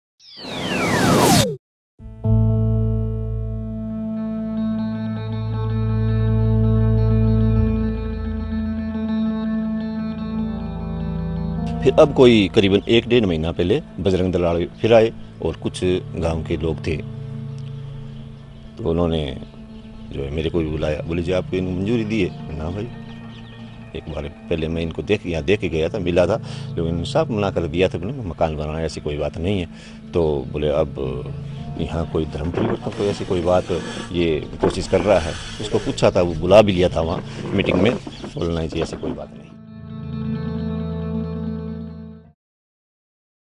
Watch: Haryana Church Vandalised, Cross Replaced With Hanuman Idol
An under-construction church was vandalised in a Haryana village on Saturday. Attacker placed Lord Hanuman’s idol in it. Police have booked 14 suspect for criminal intimidation.